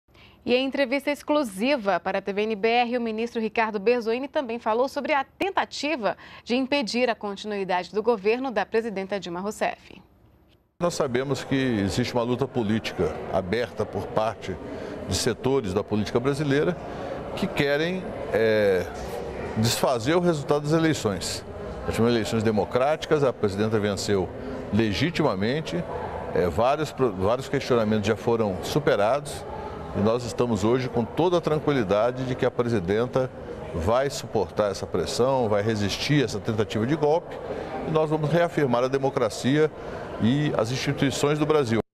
Dilma Rousseff vai resistir à tentativa de golpe, diz ministro da Secretaria-Geral
NBR NOTÍCIAS - 10.12.15: Nesta quinta-feira (10), o ministro da Secretaria-geral de Governo, Ricardo Berzoini, falou sobre a tentativa de impedimento da continuidade de governo da presidenta Dilma Rousseff. De acordo com ministro, a presidenta venceu a eleição de maneira legítima e democrática. Ricardo Berzoini afirmou ainda que Dilma vai resistir à pressão e suportar a tentativa de golpe.